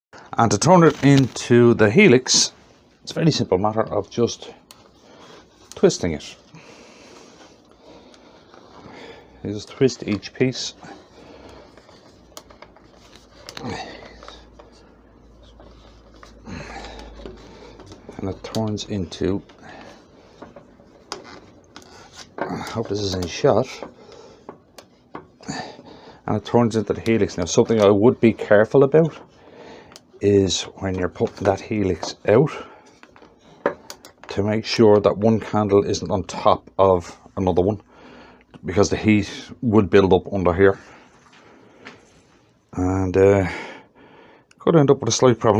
Woodturning a Transforming Helix candle holder
The short version of the transformation between tall and helix candle stick.